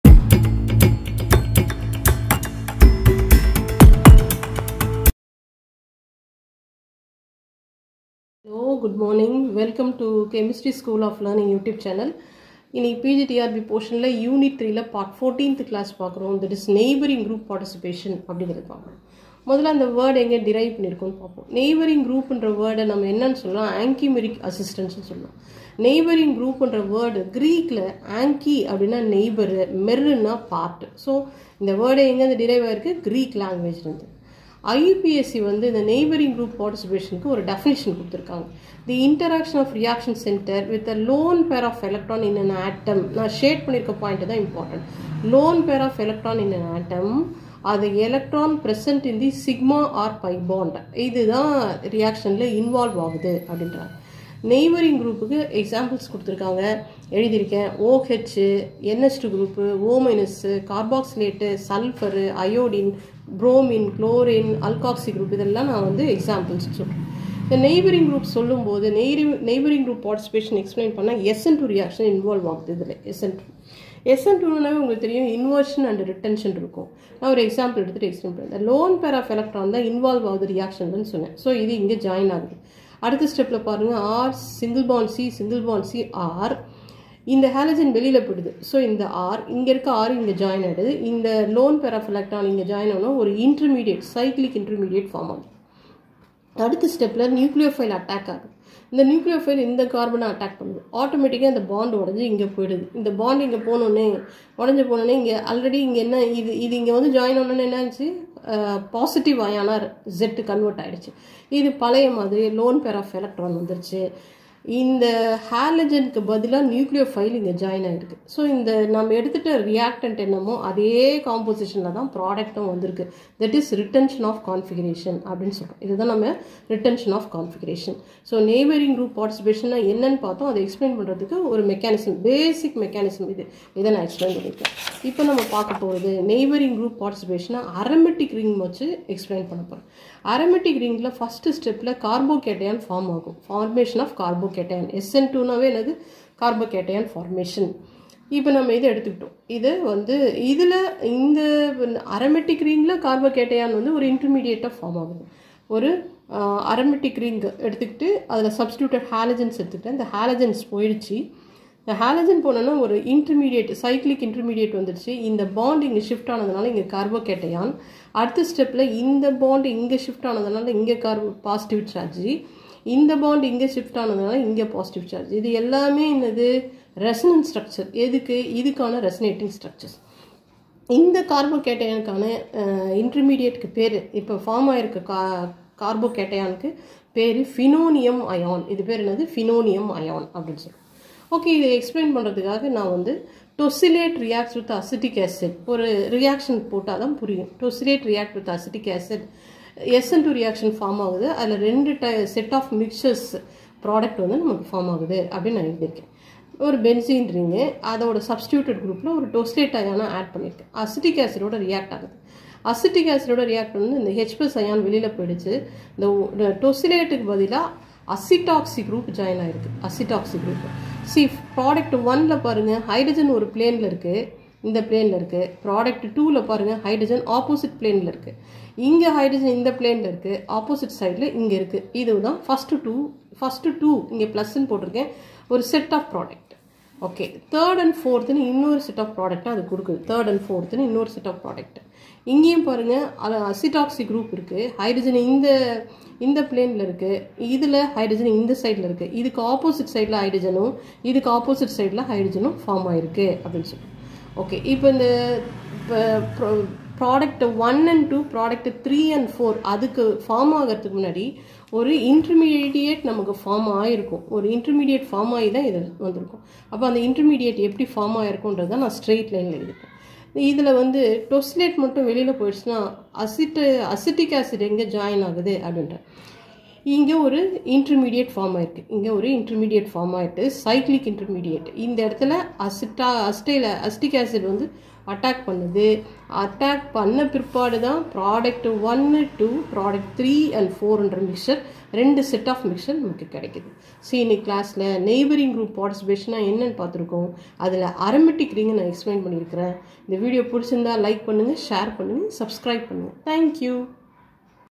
NEIGHBOURING GROUP PARTICIPATION, PG TRB CHEMISTRY, UNIT-3, PART- 14
In this video you'll learn about, "NEIGHBOURING GROUP PARTICIPATION,  PG TRB CHEMISTRY, UNIT-3, PART- 14. ANCHIMERIC ASSISTANCE, INVERSION, RETENTION  OF CONFIGURATION, PHENONIUM ION.